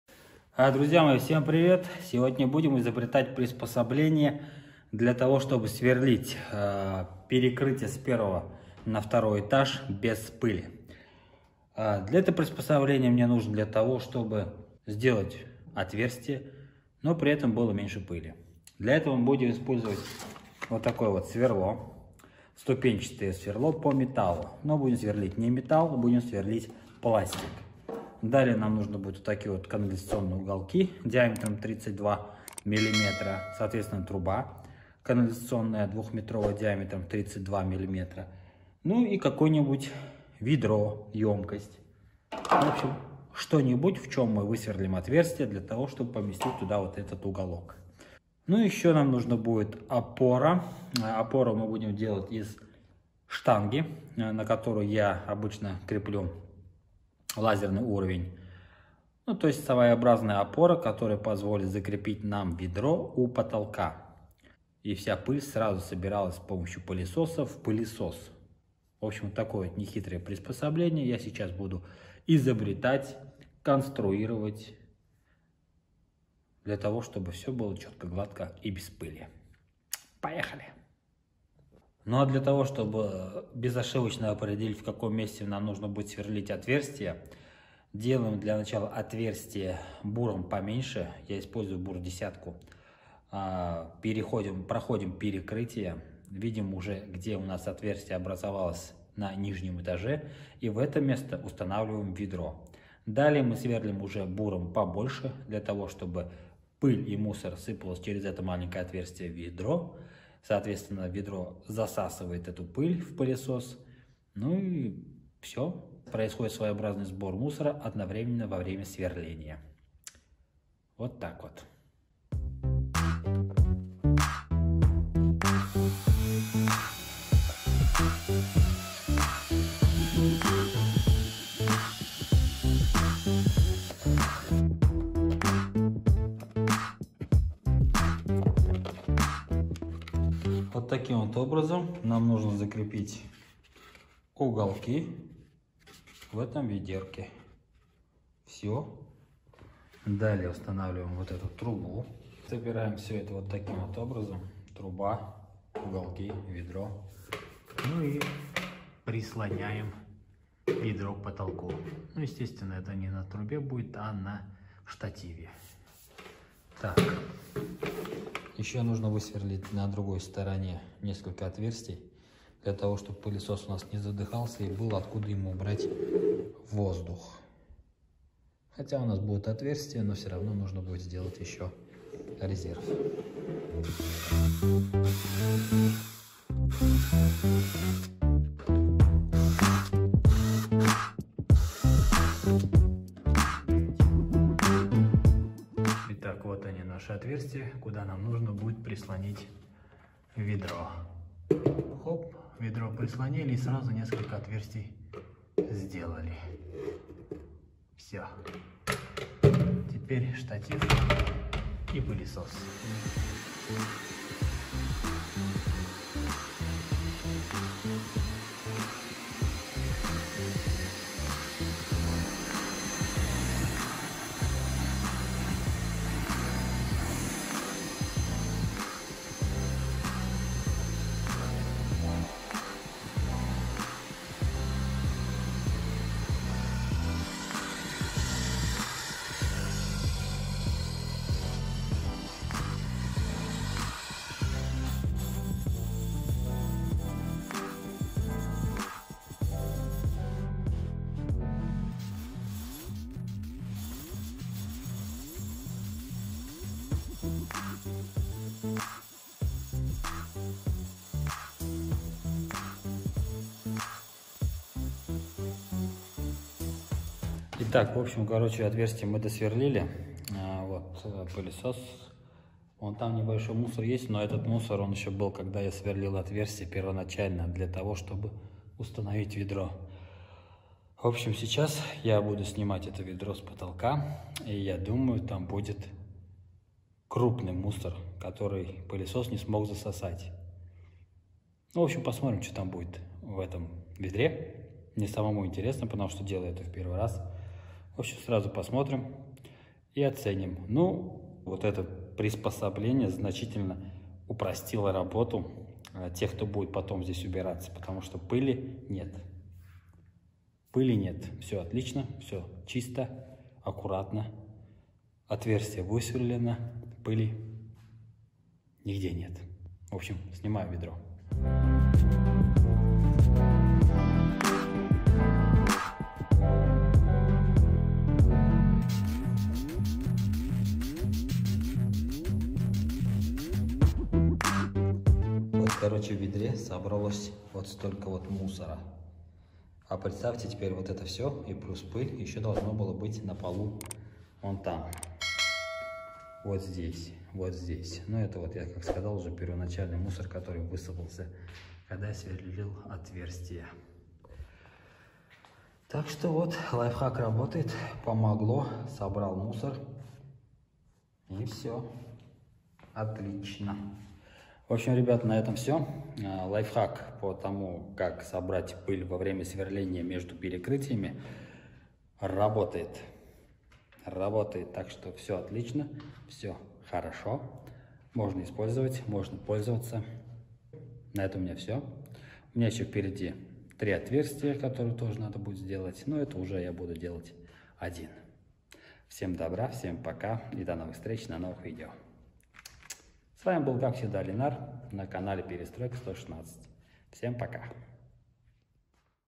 Пылеудаление своими руками? Приспособление ДЕШЕВО.
Частенько приходиться придумывать различные приспособления для обеспечения наилучшего результата.

В этот раз приспособление для сбора пыли во время сверления.

Самое главное что стоит эта приспособление очень ДЕШЕВО.
И сделать его можно всем!

Оцените идею! Как вам такая доработка в пылеудалении.

Какие способы используете вы?
Жду ваших ответов!